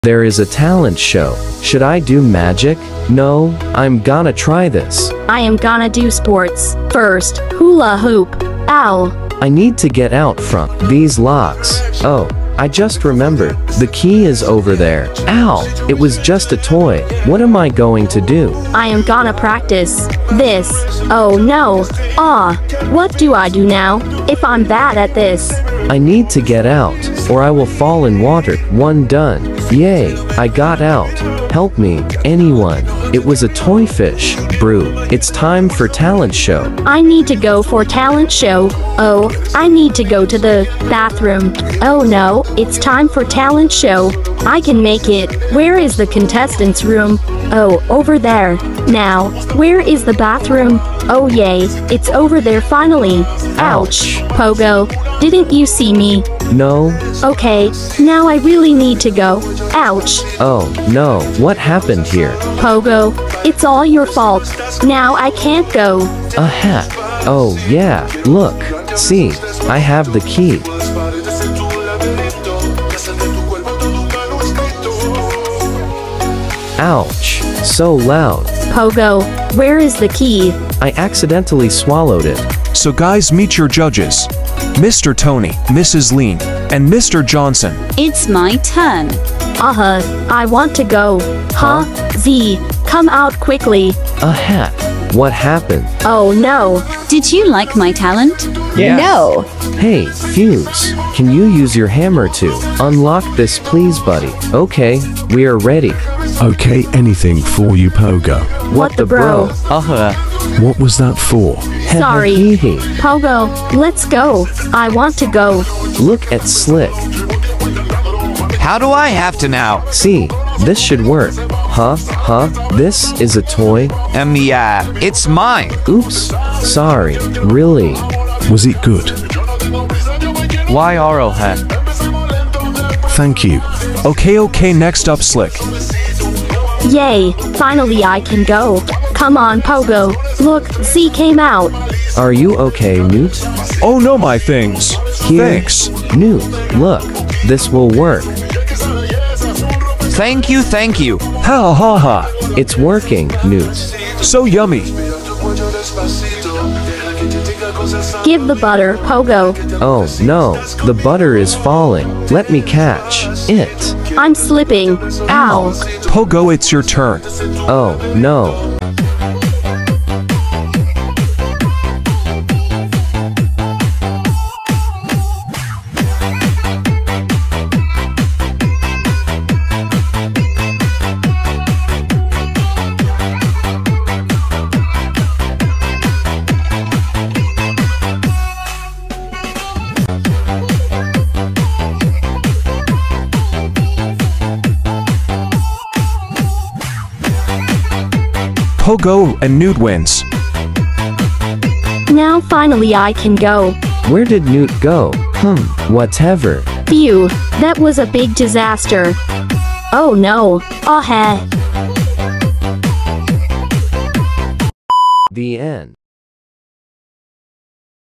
Oddbods story episode:Stuck on you*Text to speech*❤️🧡💛💚💙💜💖oddbodsepisodestories
Oddbods Text to speech stuck on you episode its about that its a oddbods talent show and pogo newt get stuck to eachother❤️🧡💛💚💙💜💖hope you all enjoy#New_Oddedit#oddbods#For_oddbods